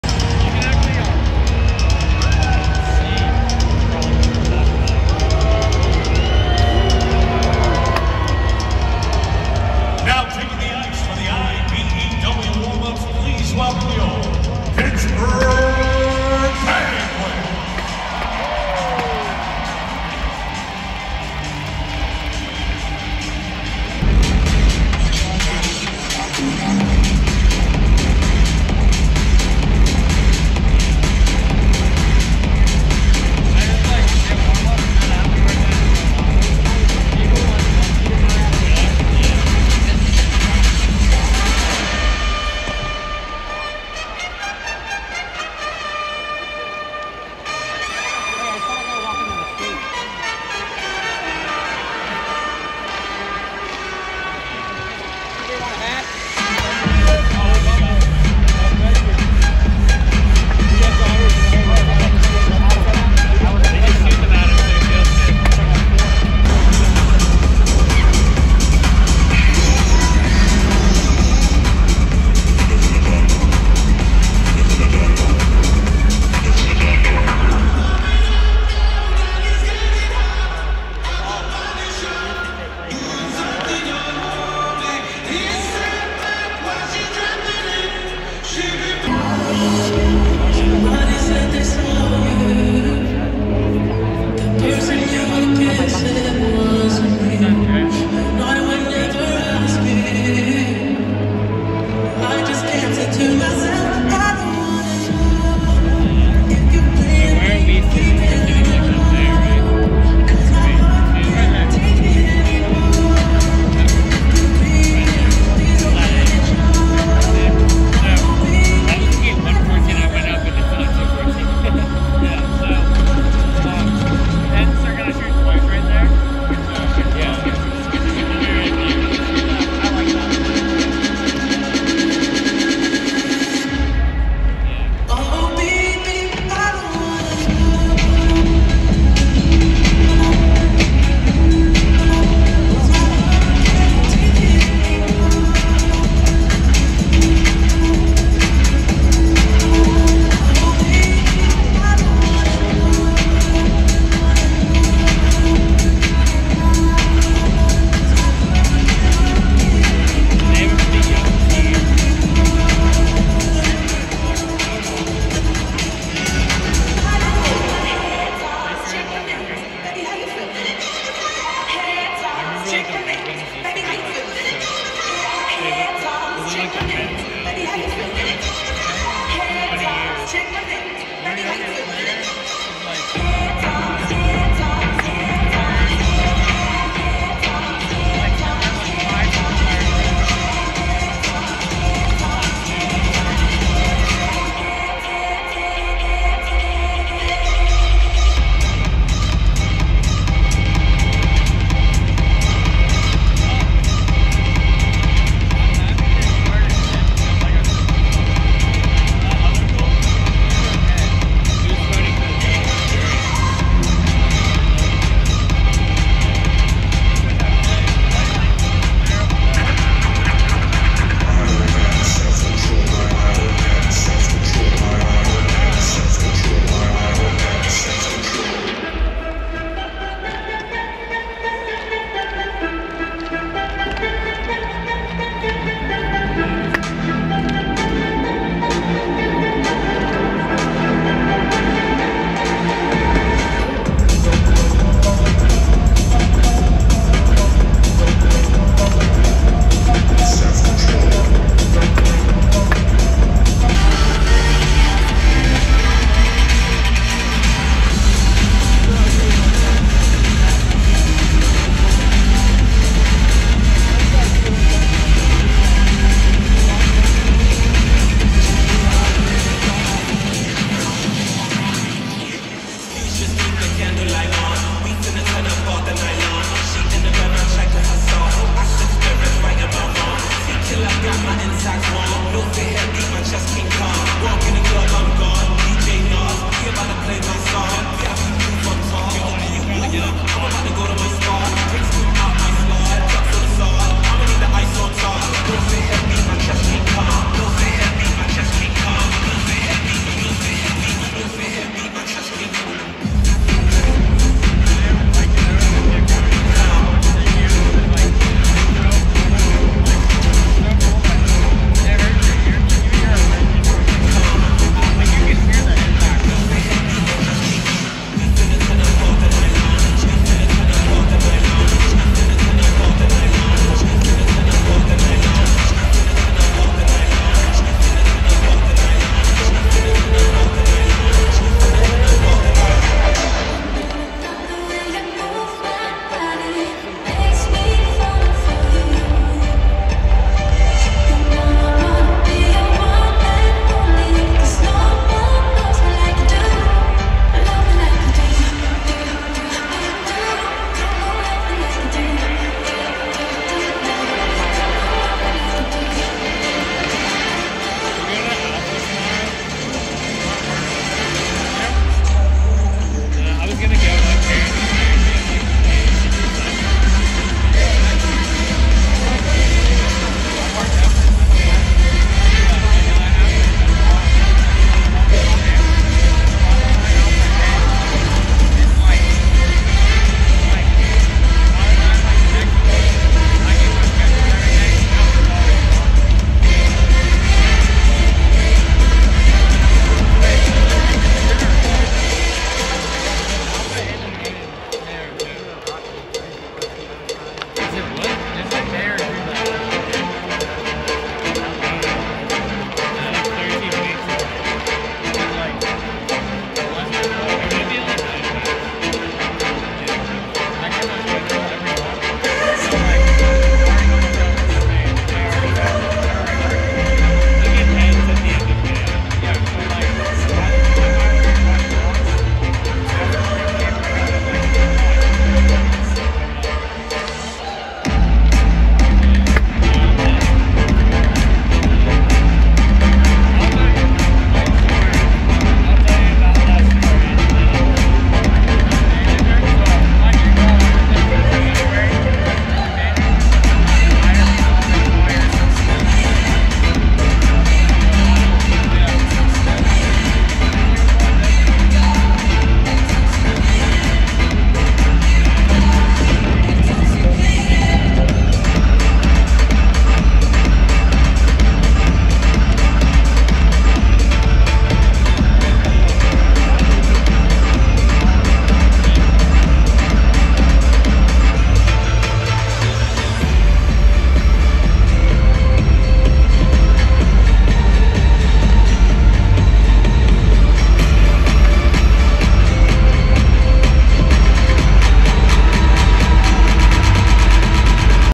Petr Mrazek & Alex Stalock Warm Ups
Petr Mrazek &  Alex Stalock warmup against the Pittsburgh Penguins at PPG Paints Arena on  April 4, 2023.